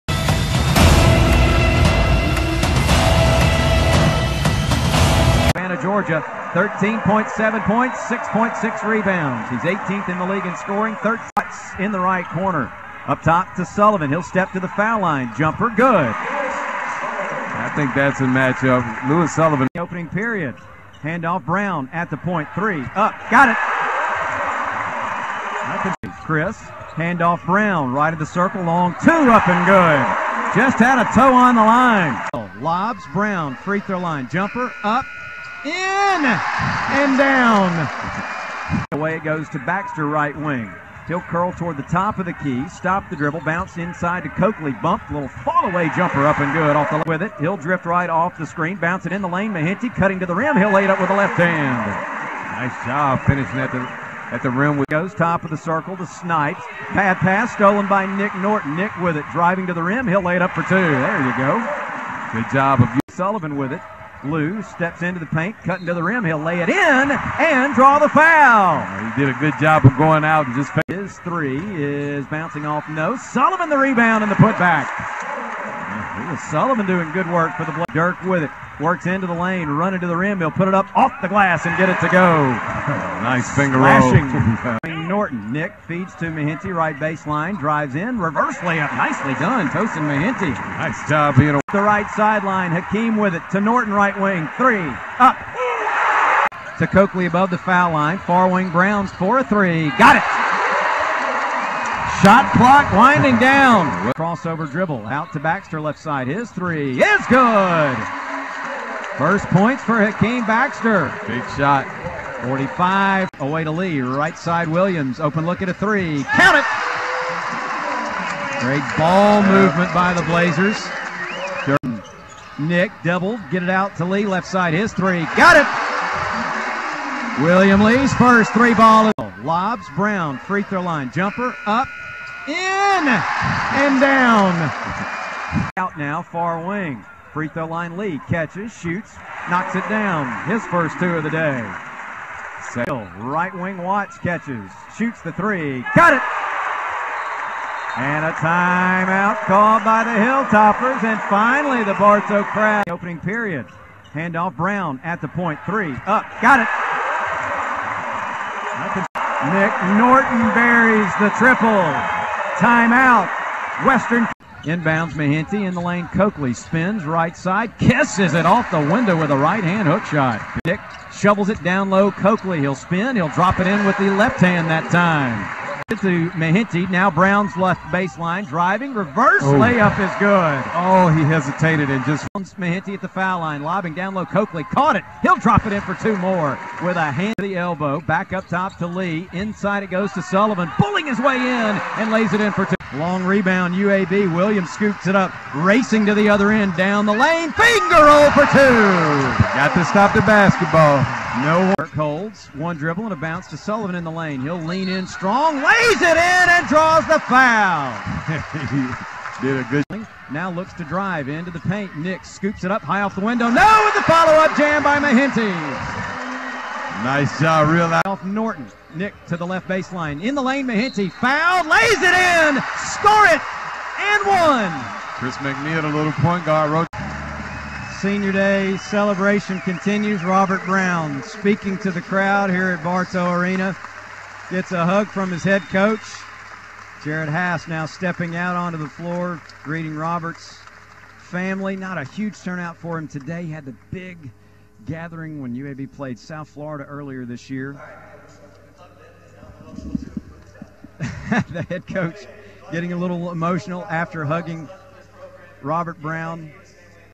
UAB Men's Baksetball vs WKU Highlights 2/27/16
Highlights from the UAB Men's basketball vs WKU game on 2/27/16 at Bartow Arena.